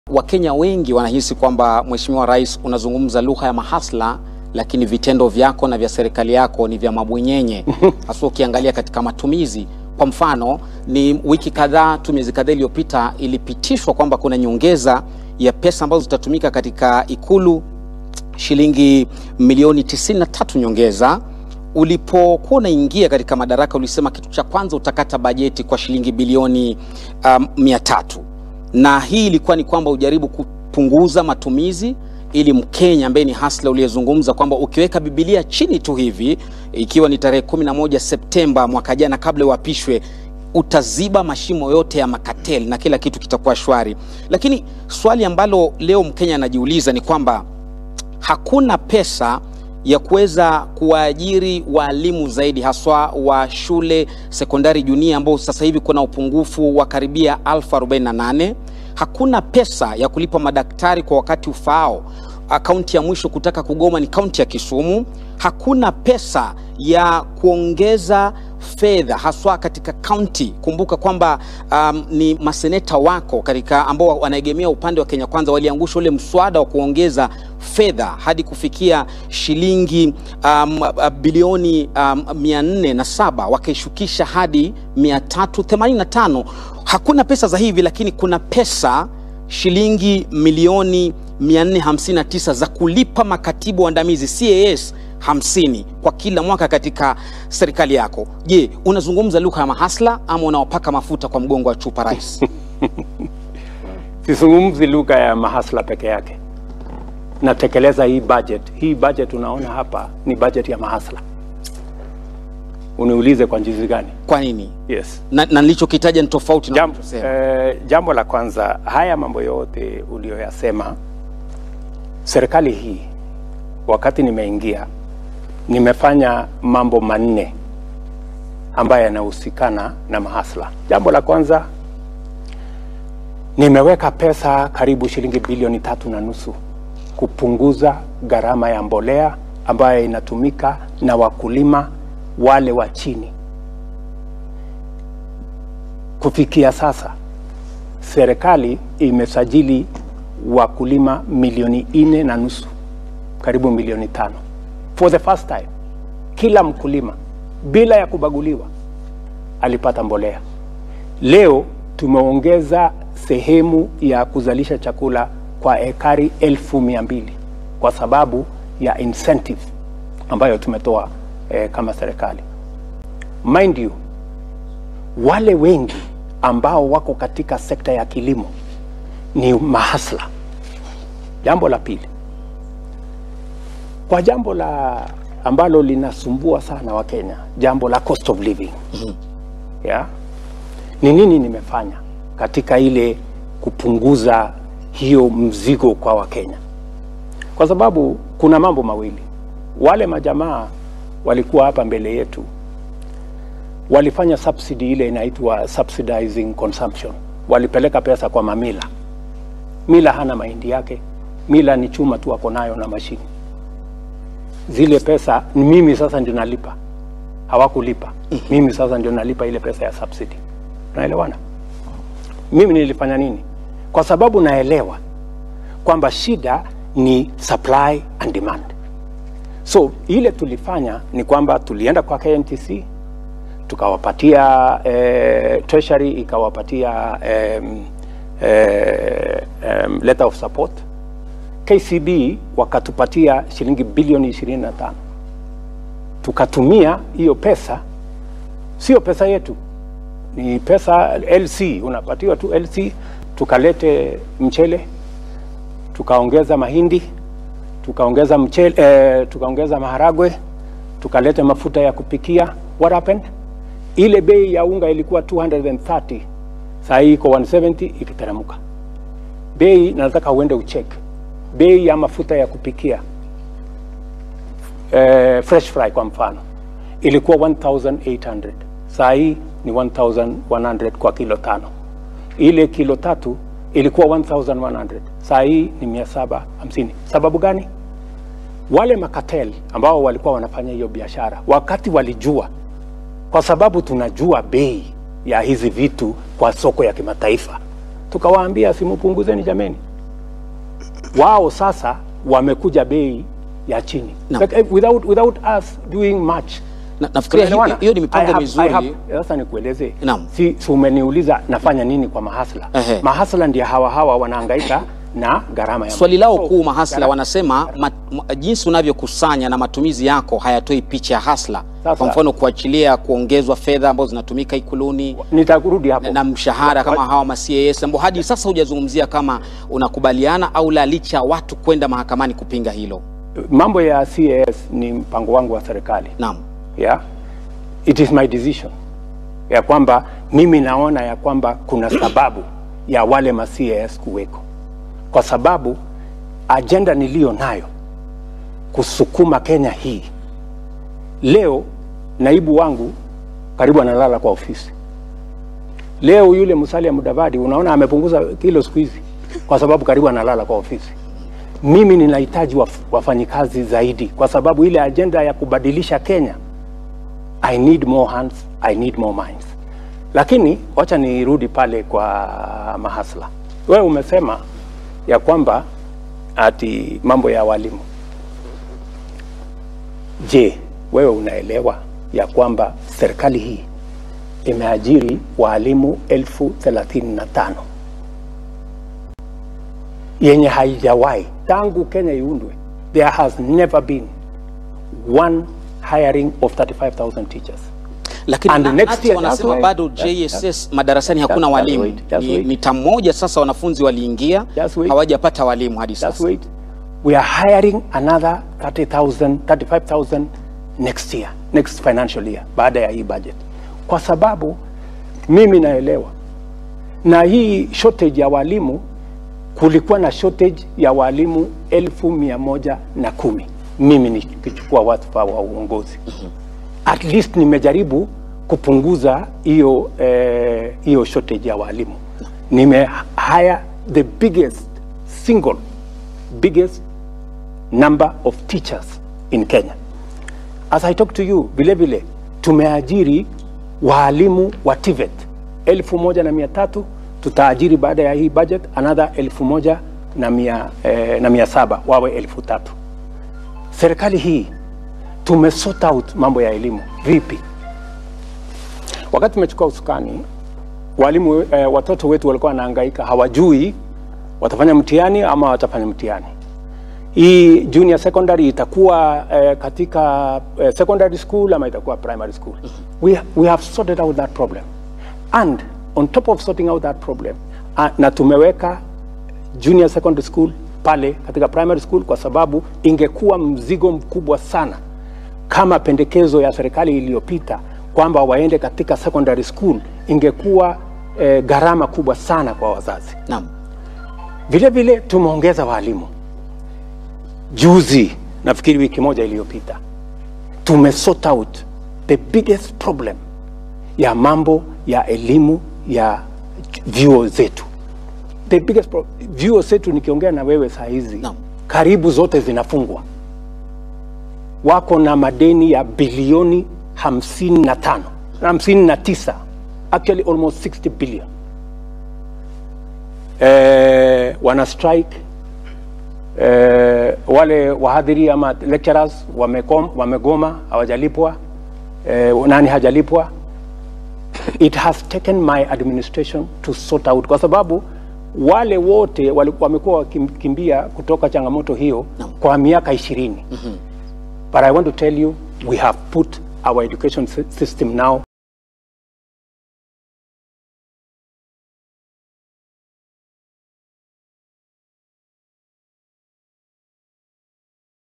Rais Ruto: Serikali yangu ni serikali ya mwananchi
Rais asema kuwa serikali imezingatia mambo muhimu kama vile mbolea, elimu na mambo mengine yanayowaguza wakenya.